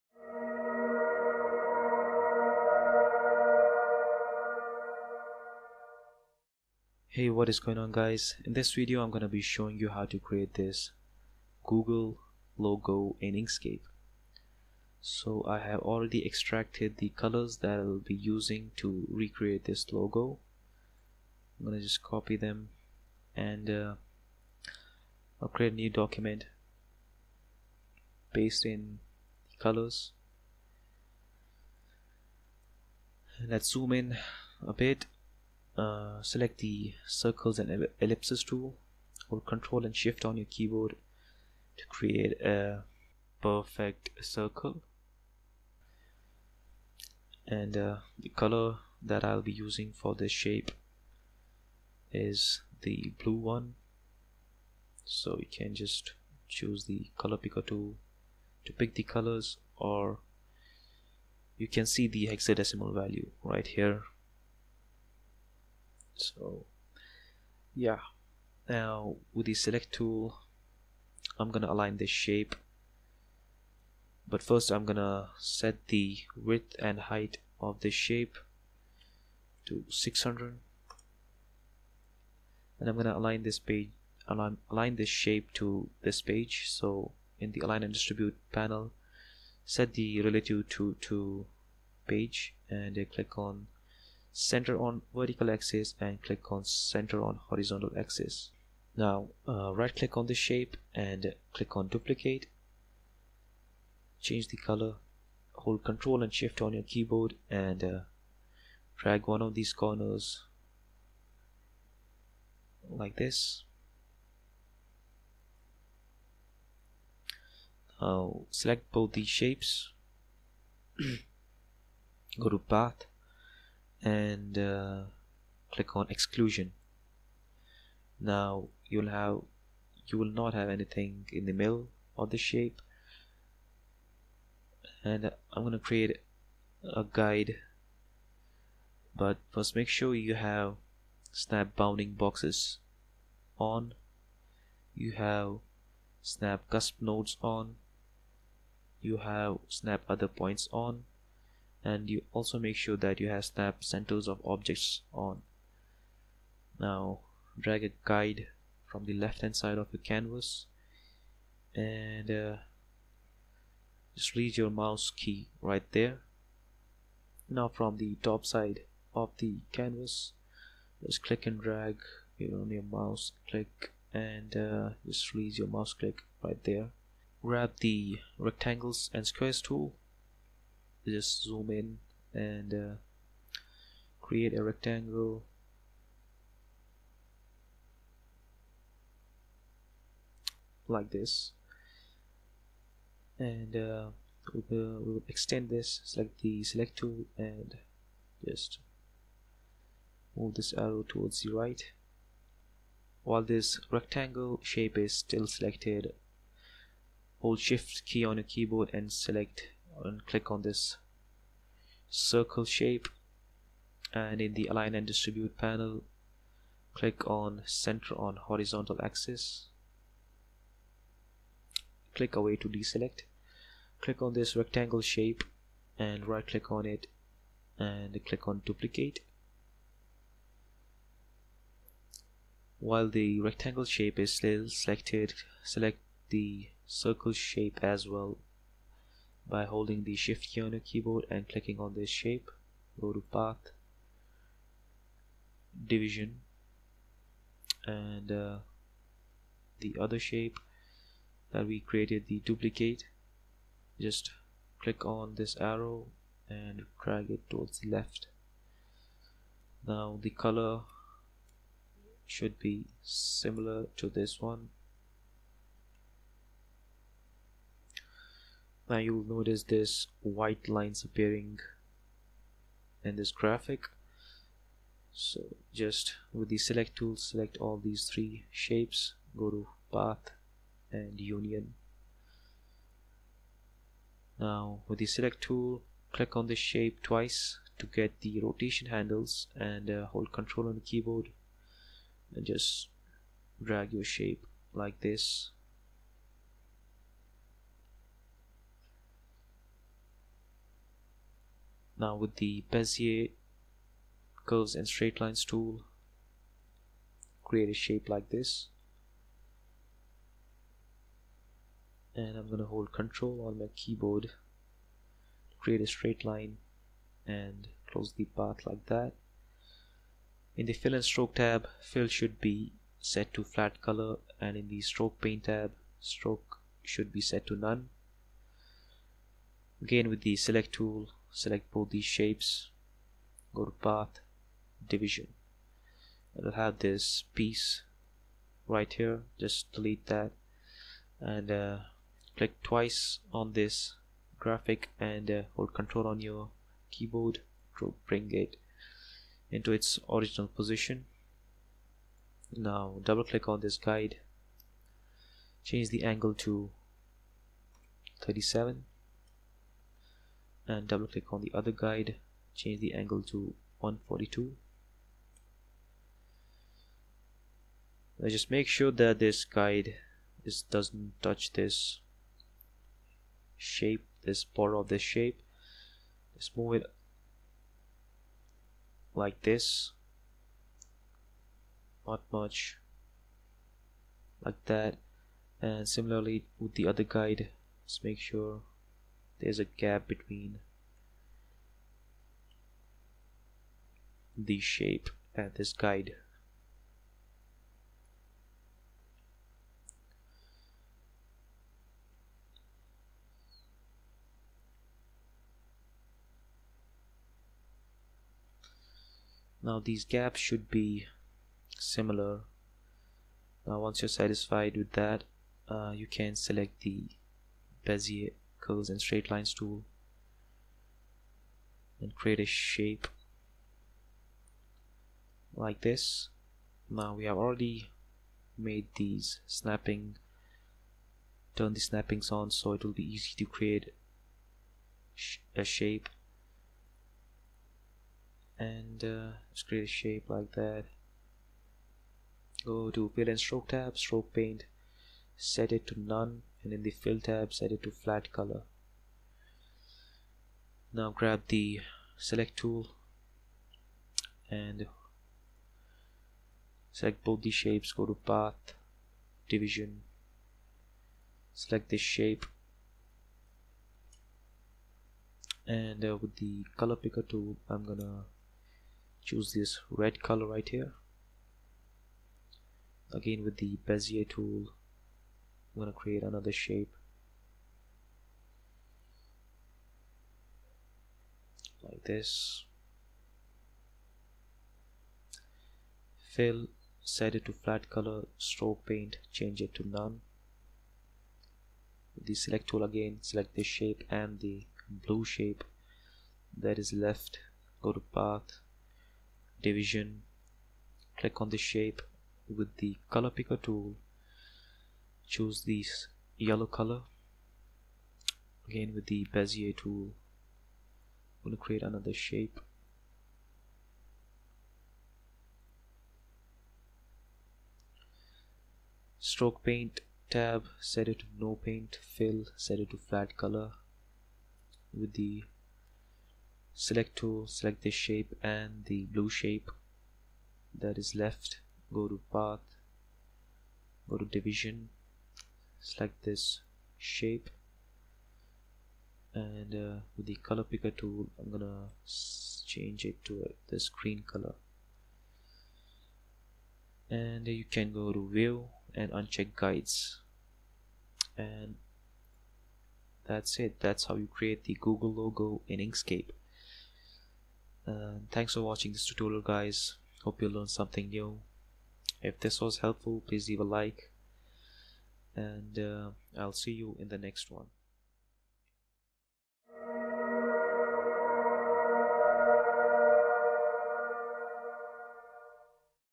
Inkscape Tutorial - Create Google Logo
It's time to create a Google logo. So colorful!

Hex color codes:
Red - e94f48ff
Yellow - f8c341ff
Green - 45b064ff
Blue - 5595f0ff

Try it with a different letter. You can use Product Sans font from Google if you don't want to create the 'G' letter yourself.